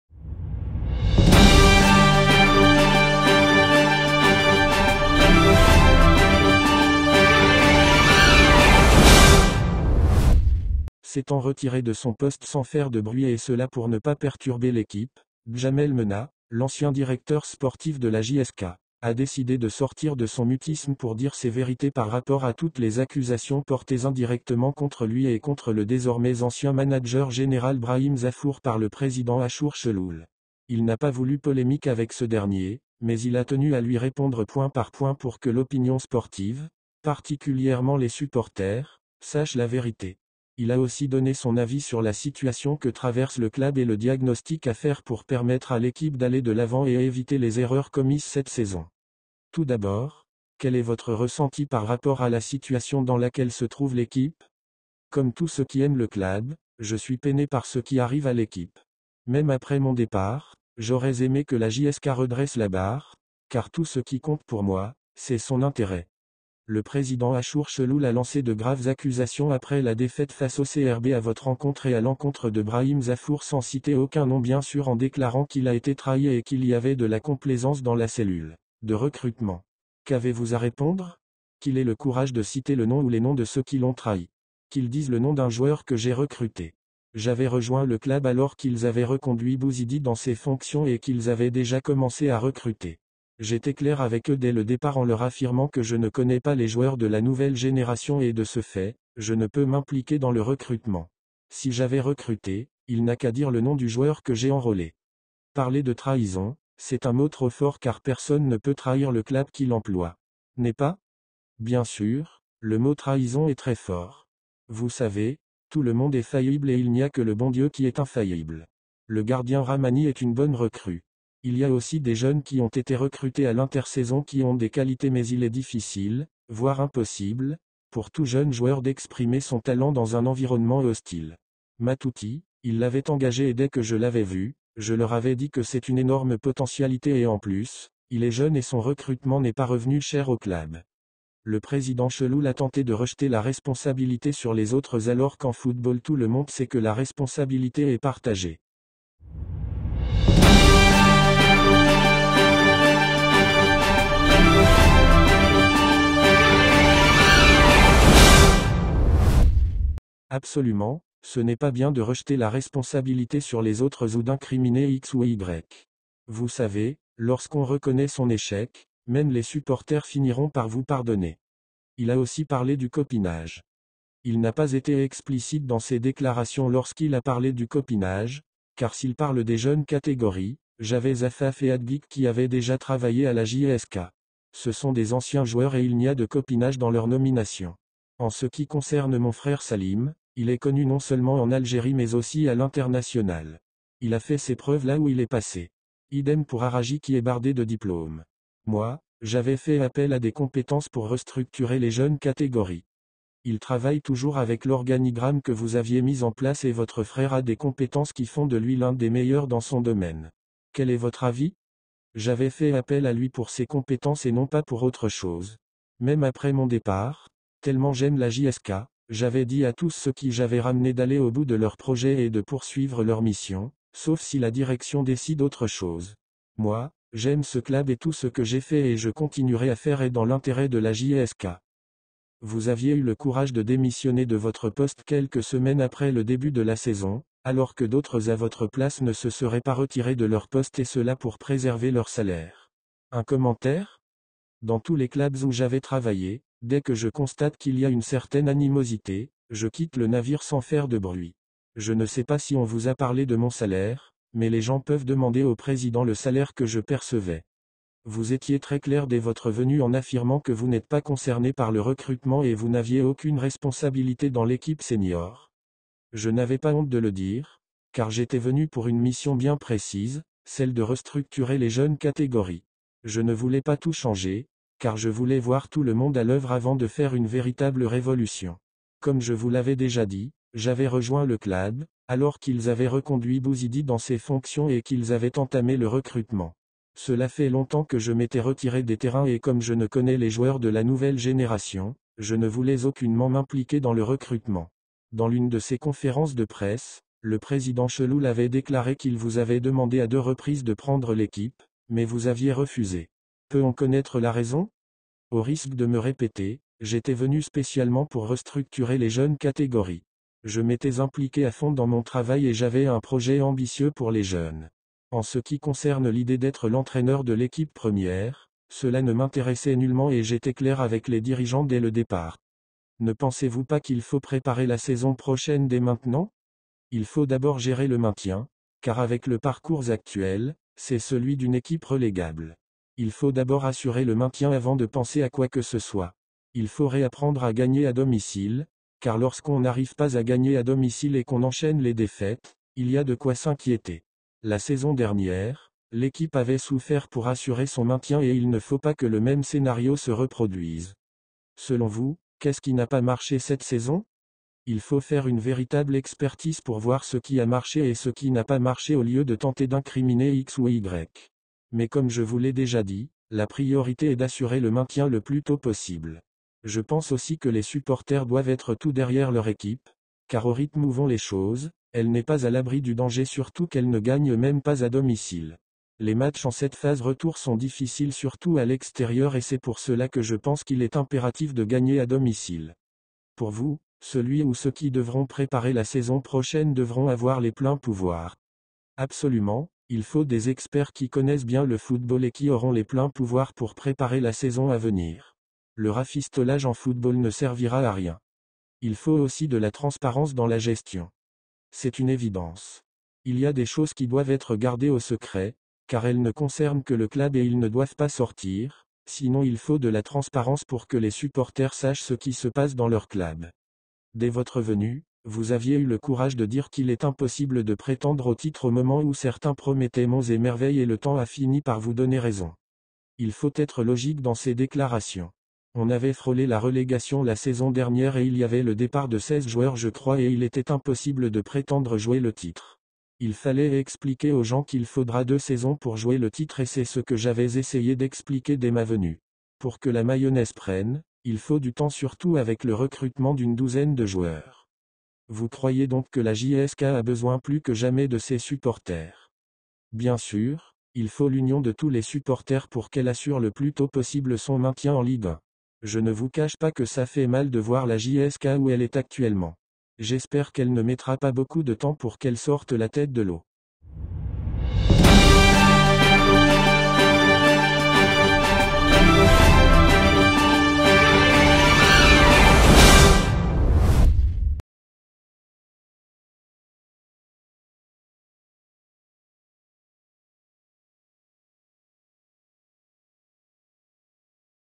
Djamel Menad Répond aux Accusations : La Vérité sur la Situation de la JSK
Découvrez les réponses de Djamel Menad aux accusations portées contre lui et Brahim Zafour par le président Achour Cheloul. Menad clarifie les malentendus et donne son avis sur la situation actuelle du club.